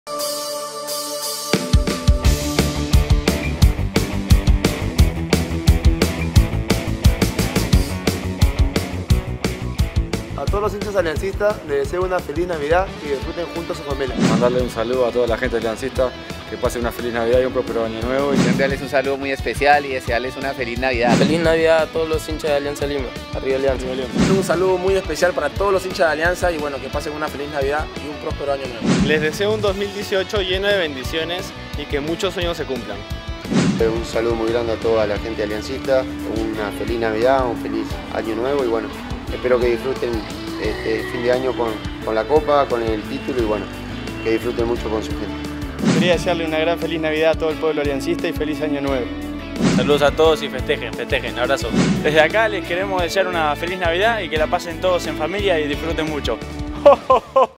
Tras ganar título, jugadores dejaron mensaje de navidad
Tras obtener el campeonato nacional 2017 jugadores enviaron mensaje efusivo previo a las fiestas navideñas.